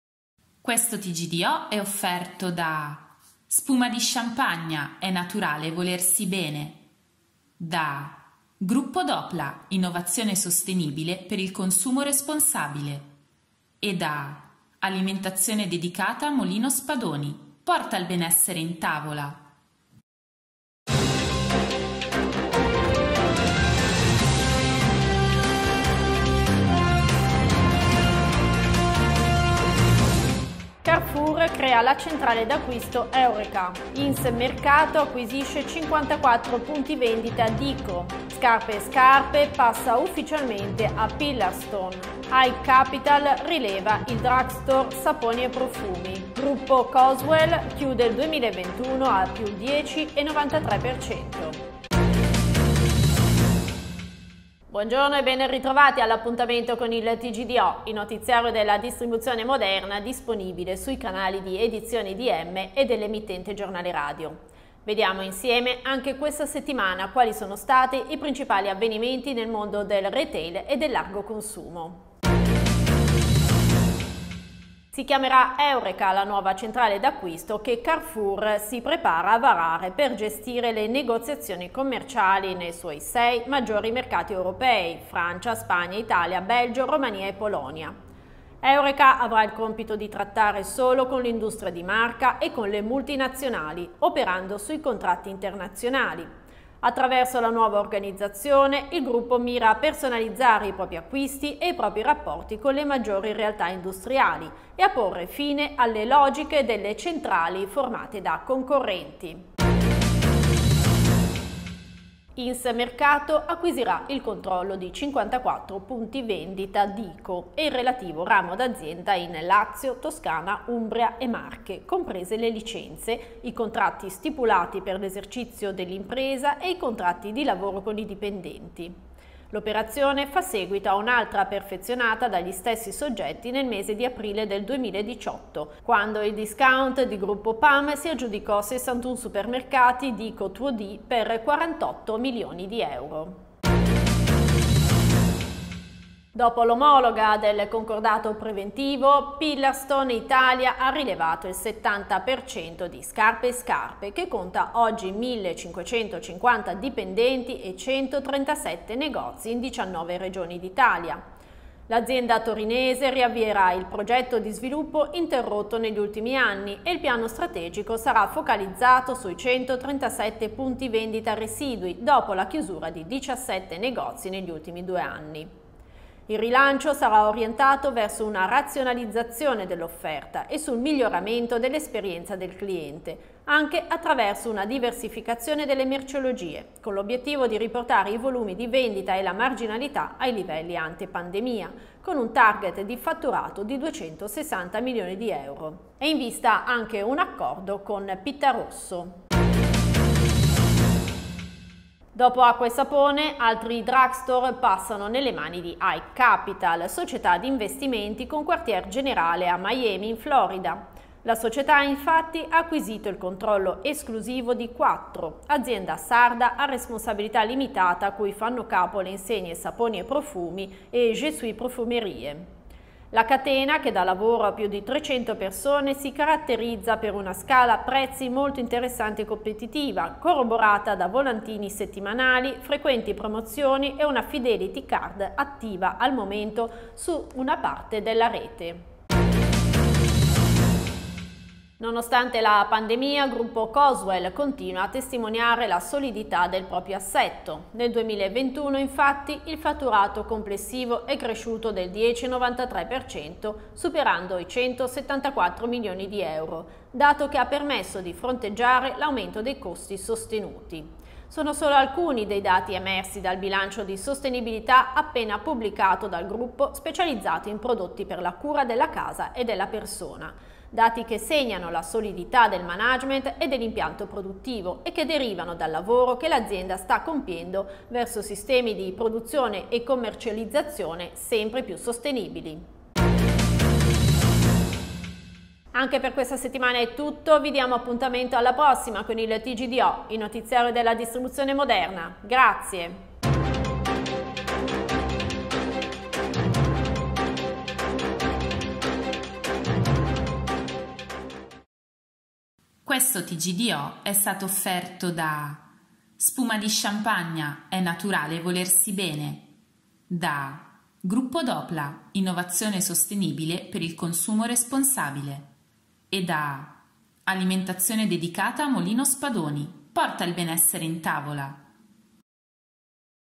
TGdo, il notiziario della distribuzione moderna. 15 settembre 2022
Le principali notizie della settimana dal 9 al 15 settembre 2022.